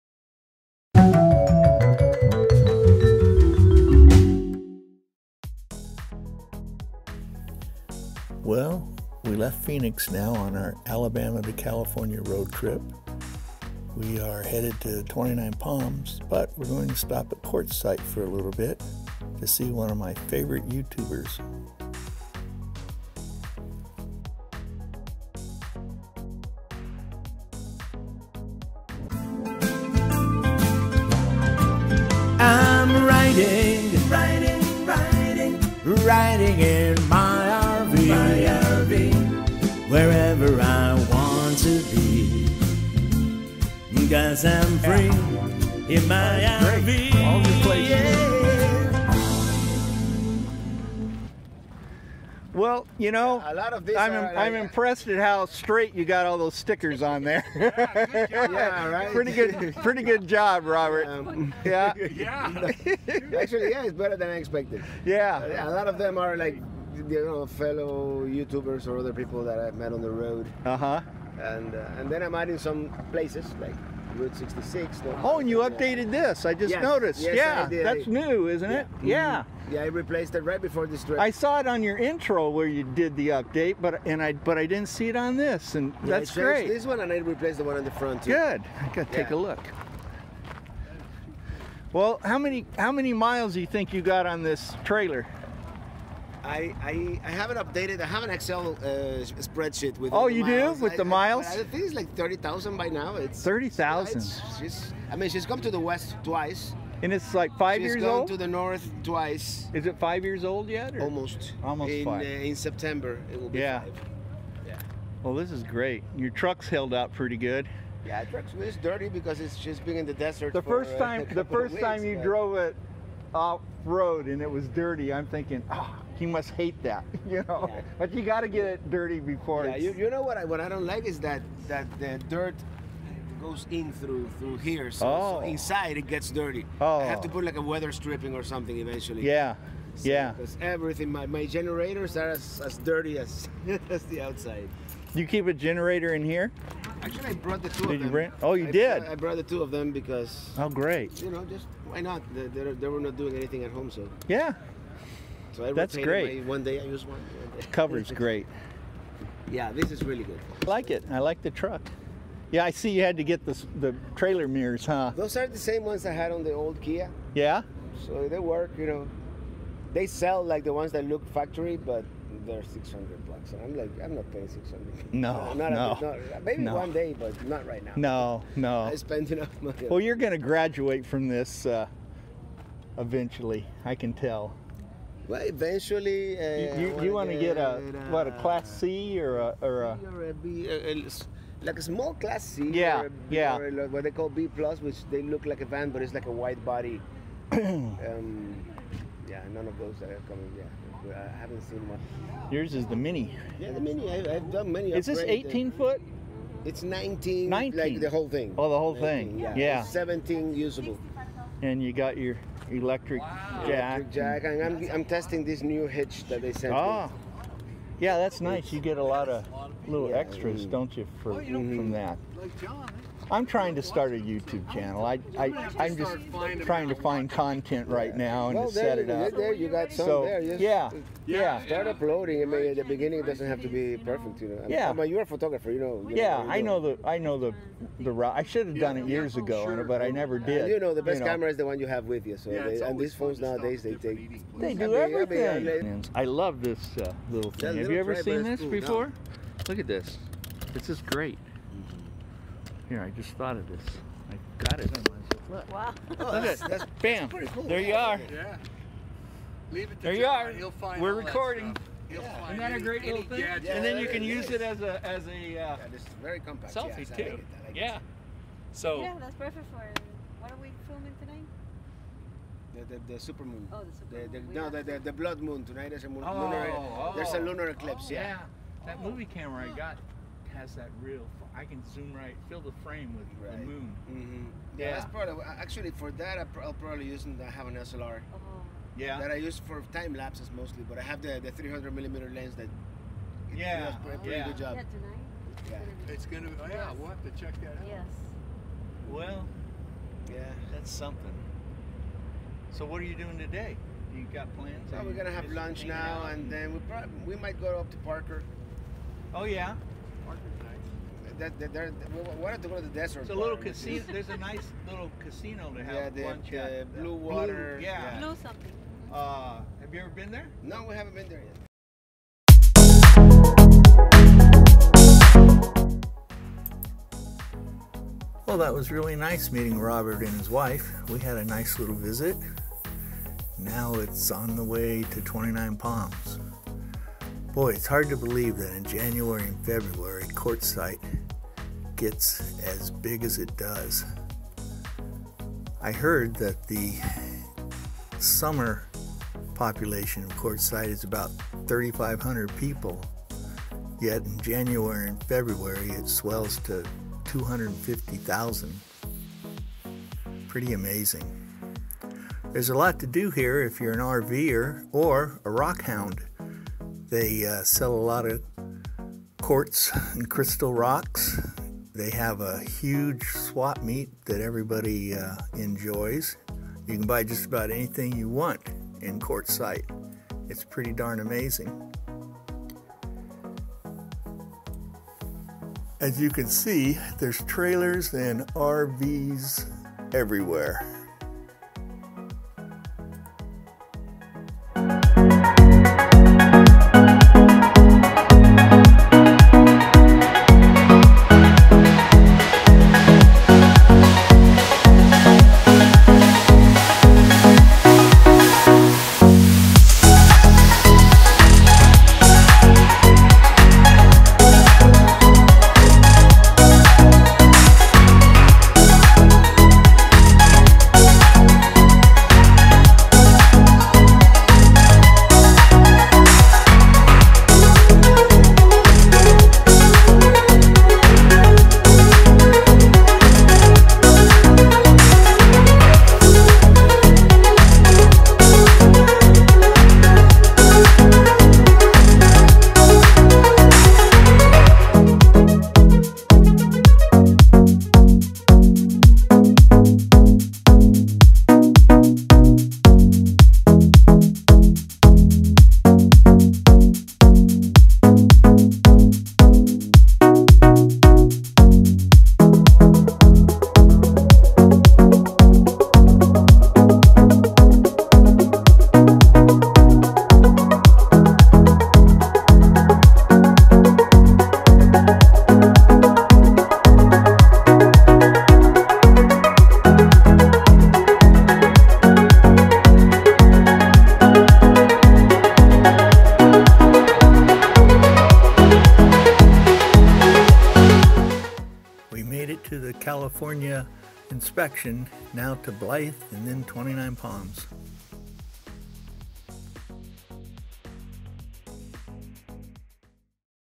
RV road trip Alabama to California Quartzite
RV road trip Alabama to California
Leaving Phoenix Arizona on the way to 29 Palms California quick stop in Quartzsite to see one of my favorite YouTubers!